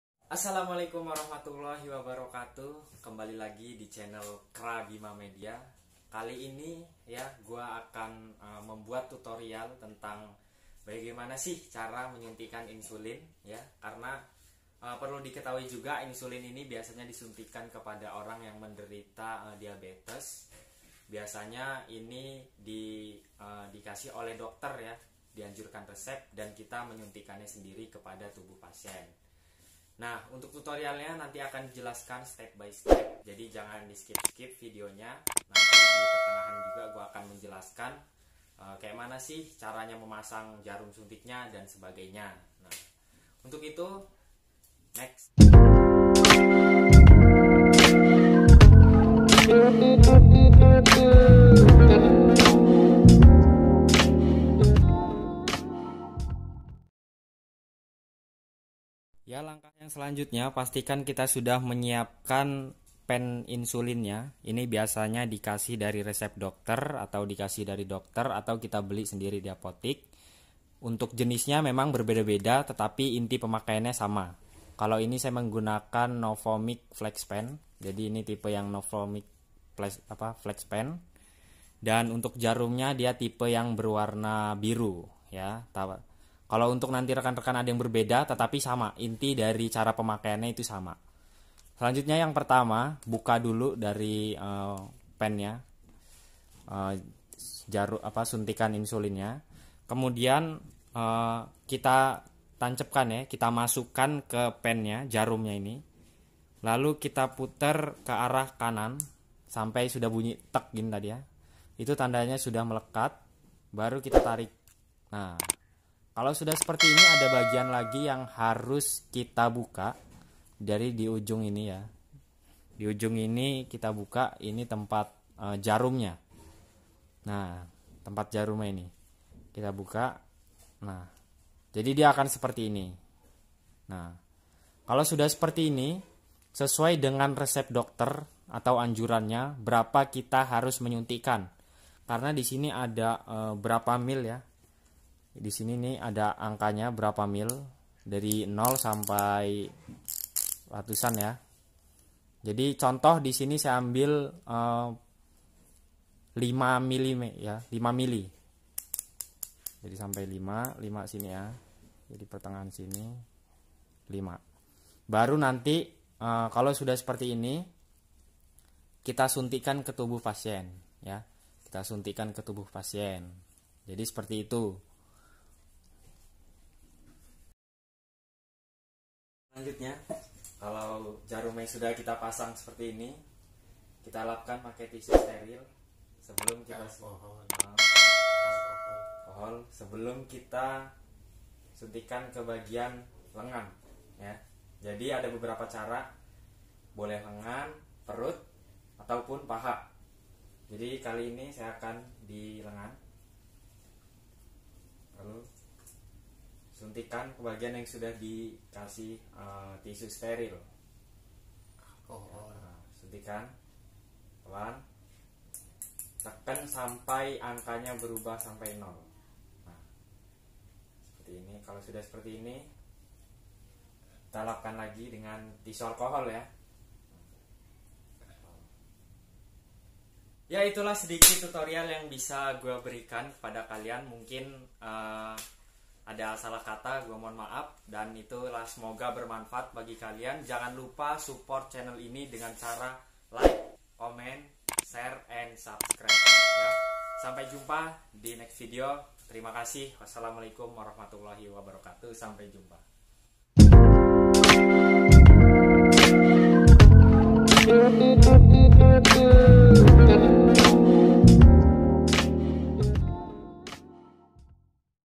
TUTORIAL SUNTIK INSULIN simpel langsung bisa
Hallo gaes

Di video ini aku membagikan tutorial cara suntik insulin yg di tunjukan untuk penderita diabetes dan perwat pasien nya ataupun bagi kalian yang ingin mempelajari bidang kesehatan

Editor @mcakrabima
Akun youtube @qrabima

Yuk dukung chanel ini
Supaya terus bisa membagikan tips dan tutorial menarik

Coment pendapat kalian
Trimakasih